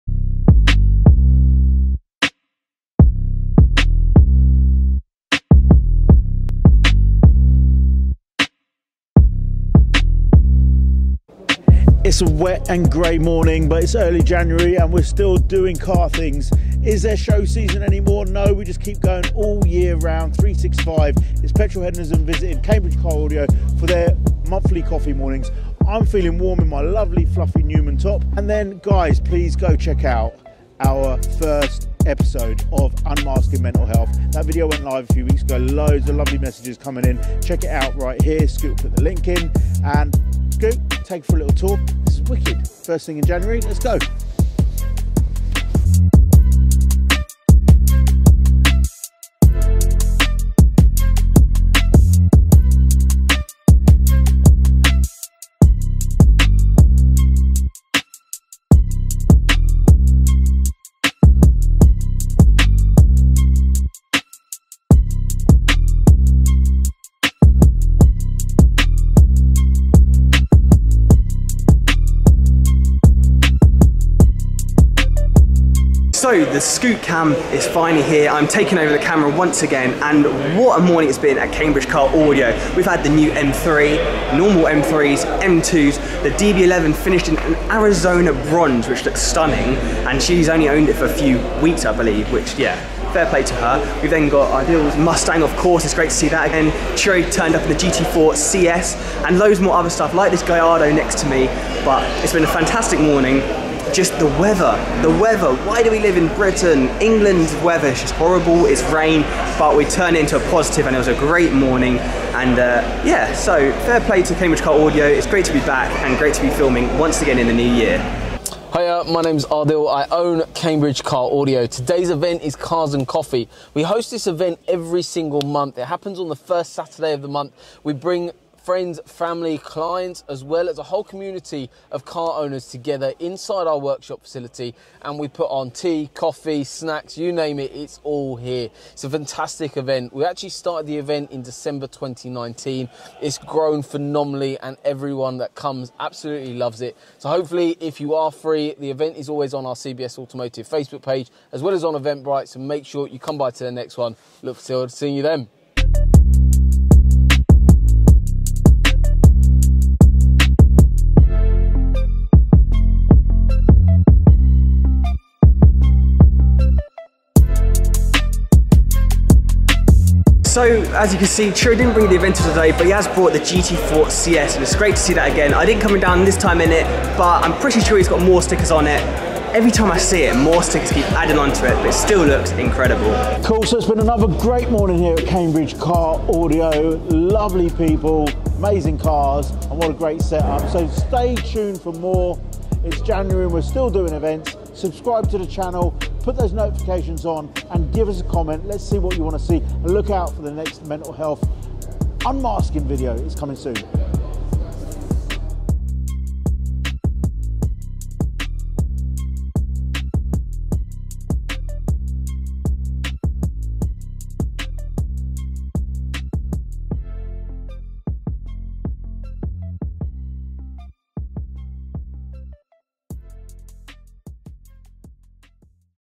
The first CARS and COFFEE 2022!
What is up everyone and welcome back to a brand new vlog here on the Petrolheadonism YouTube Channel!

Yesterday me and Ciro headed down to @CambridgeCarAudio for their first cars and Coffee of 2022! One that they do every month and was great to go along yesterday with a fantastic array of sports and supercars!

Once again a massive thank you to CCA for putting on a great morning, make sure you go and check out their YouTube channel